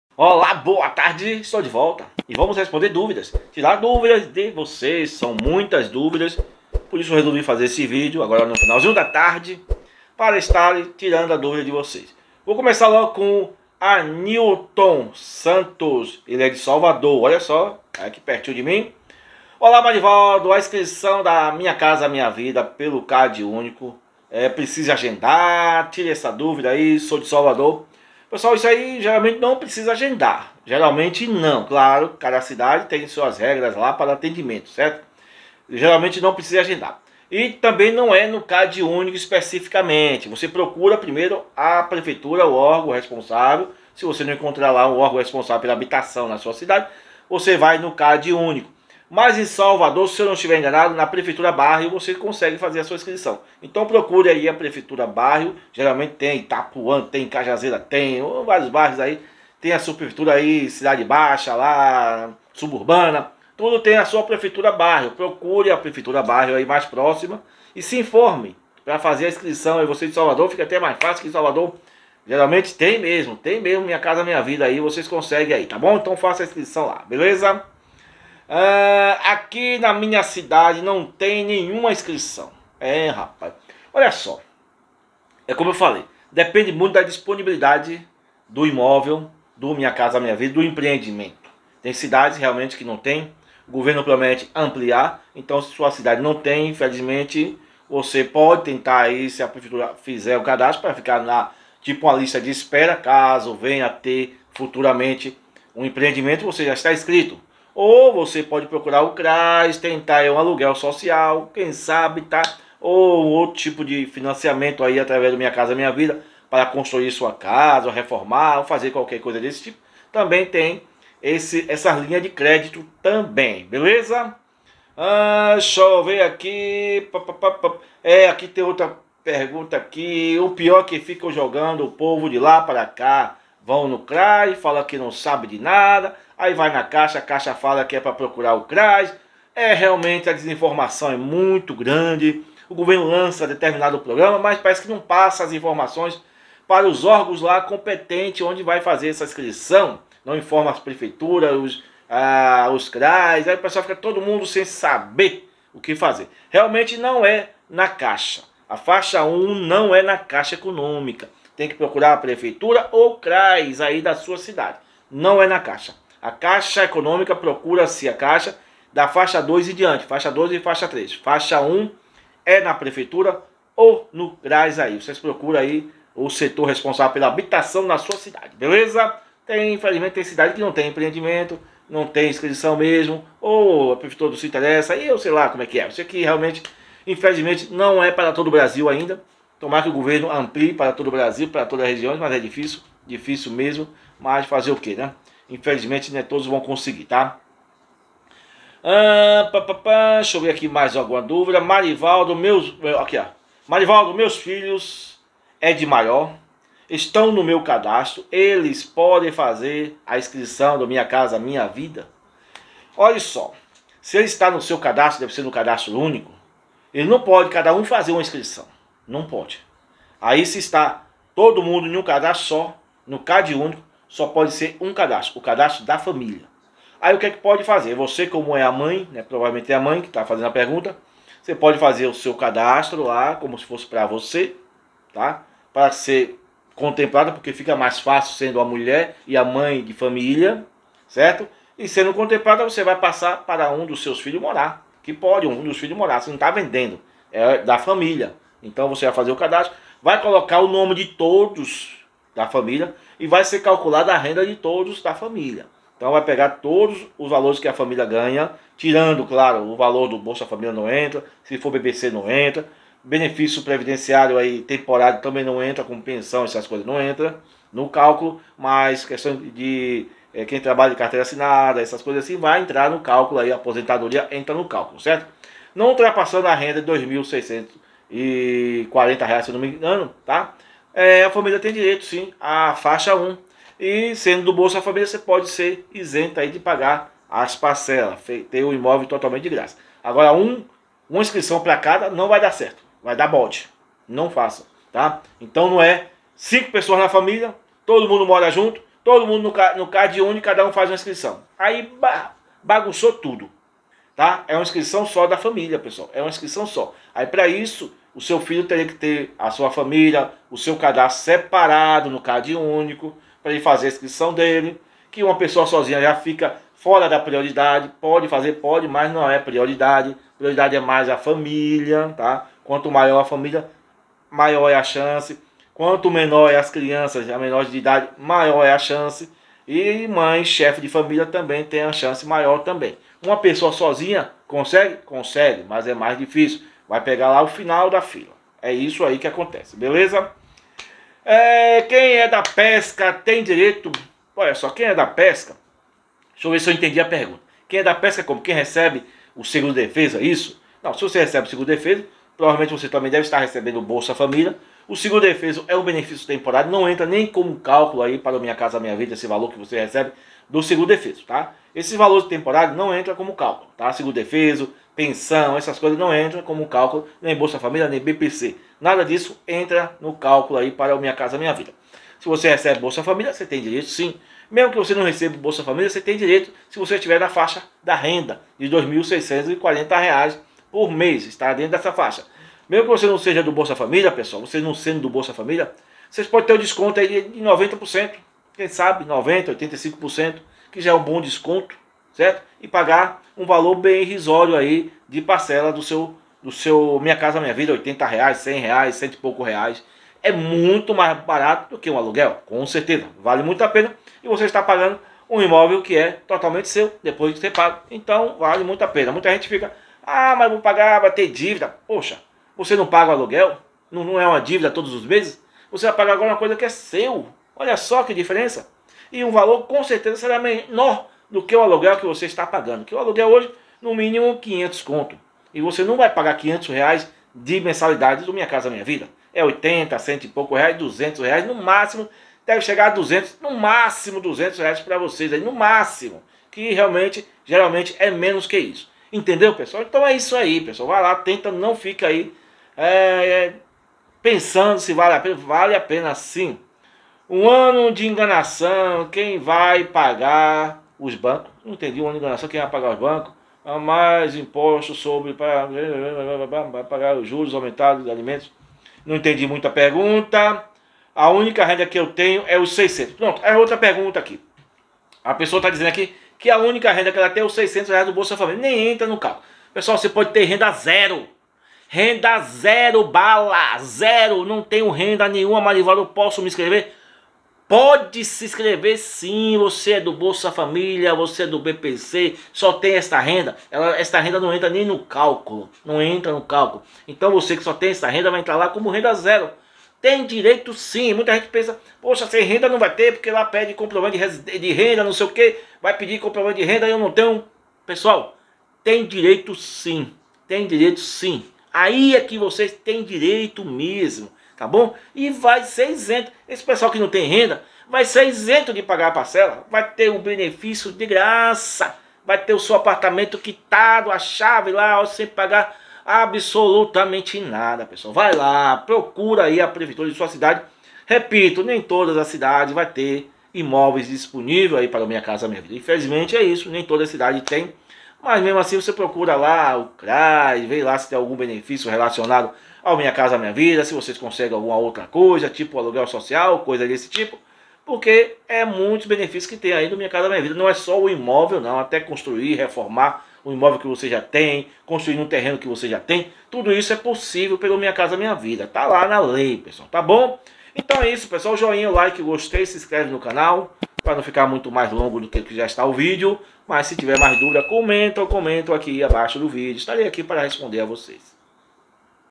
BOLSA FAMÍLIA - SEM RENDA TEM DIREITO AO MINHA CASA MINHA VIDA? TIRE ESSA E OUTRAS DÚVIDAS.
Bolsa Família, quem não tem renda ou só tem o valor do Bolsa Família tem direito ao MINHA CASA MINHA VIDA?

A resposta é sim, é ainda tem direito a inscrição sem precisar pagar as parcelas, se a pessoa for do Bolsa Família ou BPC.

Notícias, informações e opinião sobre os assuntos mais relevantes Auxílio Brasil e Empréstimo Consignado

- DEUS ABENCÕE A TODOS!